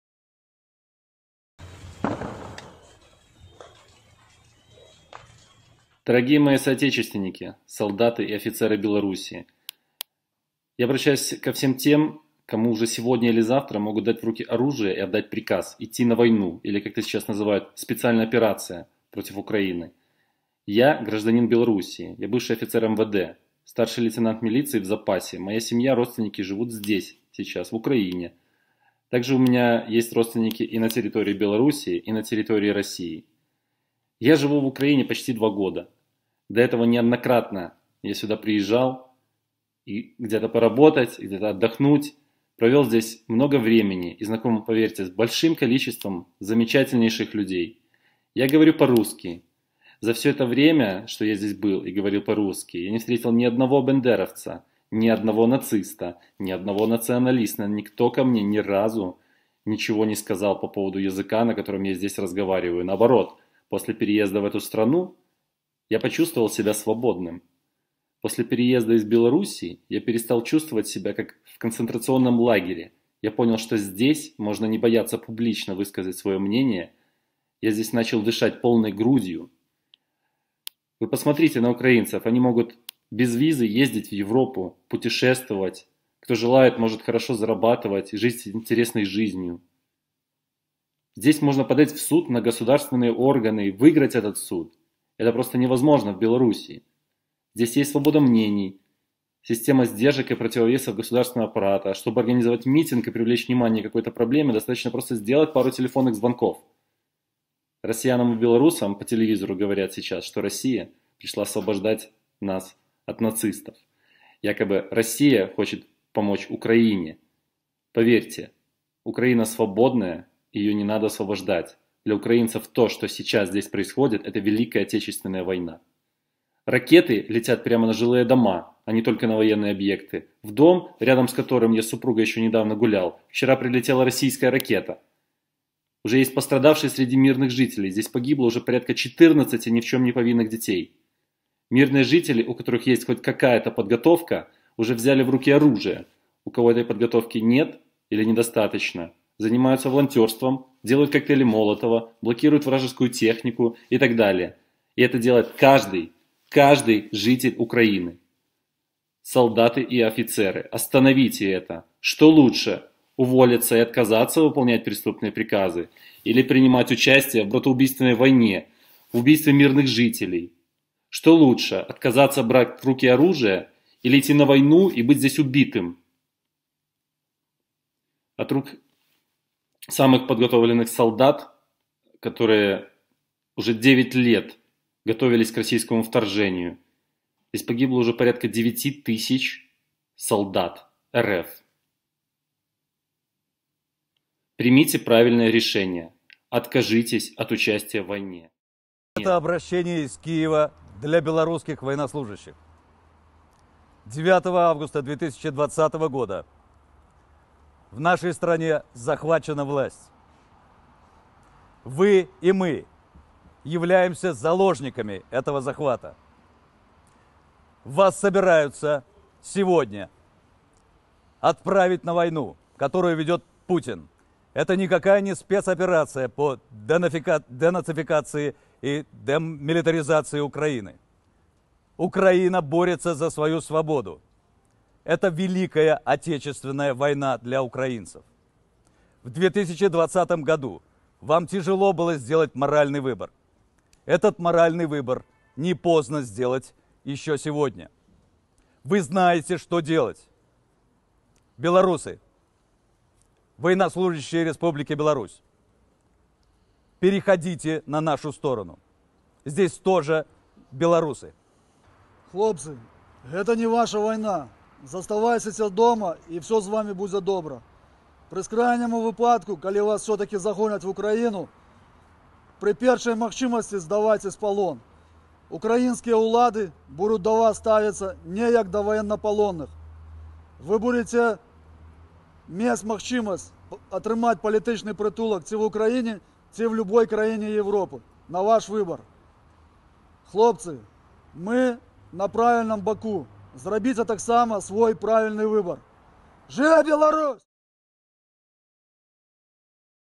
БЕЛАРУСЬ не ввязывайся в войну
2 года назад Украина стала убежищем для более 5000 белорусов, которые нашли в себе силы выступить против диктатуры и вранья. Сегодня белоруская власть направляет свою армию убивать тех, кто поддержал вас в сложное время.

Мы обращаемся ко всем матерям, жёнам, дочерям Беларуси: не отпускайте своих мужчин на поле боя. Имперские амбиции не стоят ни капли крови, ни одной жизни. «Сохраните свою семью!»

Мы обращаемся к солдатам: это не ваша война, а заказ на уничтожение украинского народа. Пересек границы Украины с оружием, вы станете причастны к убийствам мирного населения: детей, женщин, пожилых. Неужели это то, ради чего вы отдавали присягу?

Мы обращаемся к активистам Беларуси: выходите на улицы пока не поздно, пока ваша экономика ещё имеет шансы выстоять, пока цивилизационный мир не отвернулся от вас.